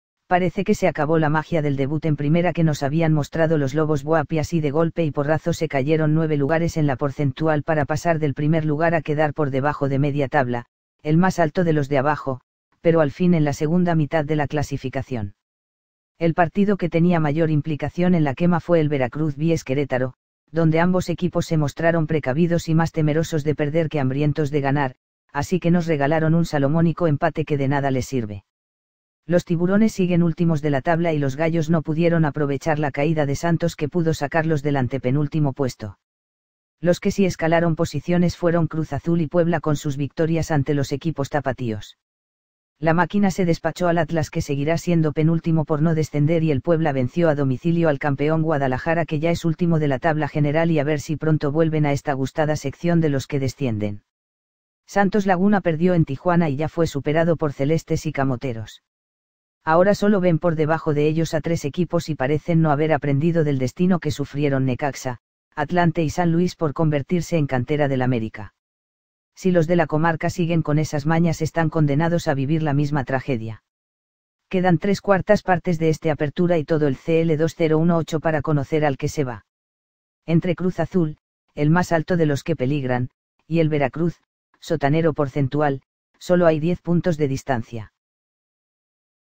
Así se quedo el descenso en la Liga Mx tras la J5!
Parece que se acabó la magia del debut en Primera que nos habían mostrado los Lobos BUAP y así de golpe y porrazo se cayeron 9 lugares en la porcentual para pasar del primer lugar a quedar por debajo de media tabla, el más alto de los de abajo, pero al fin en la segunda mitad de la clasificación.

El partido que tenía mayor implicación en la quema fue el Veracruz vs Querétaro, donde ambos equipos se mostraron precavidos y más temerosos de perder que hambrientos de ganar, así que nos regalaron un salomónico empate que de nada les sirve. Los Tiburones siguen últimos de la tabla y los Gallos no pudieron aprovechar la caída de Santos que pudo sacarlos del antepenúltimo puesto.

Los que sí escalaron posiciones fueron Cruz Azul y Puebla con sus victorias ante los equipos tapatíos. La Máquina se despachó al Atlas que seguirá siendo penúltimo por no descender y el Puebla venció a domicilio al campeón Guadalajara que ya es último de la tabla general y a ver si pronto vuelven a esta gustada sección de los que descienden.

Santos Laguna perdió en Tijuana y ya fue superado por Celestes y Camoteros. Ahora sólo ven por debajo de ellos a tres equipos y parecen no haber aprendido del destino que sufrieron Necaxa, Atlante y San Luis por convertirse en cantera del América. Si los de la Comarca siguen con esas mañas están condenados a vivir la misma tragedia.

Quedan tres cuartas partes de este Apertura y todo el Cl-2018 para conocer al que se va. Entre Cruz Azul (el más alto de los que peligran) y el Veracruz (sotanero porcentual) sólo hay diez puntos de distancia.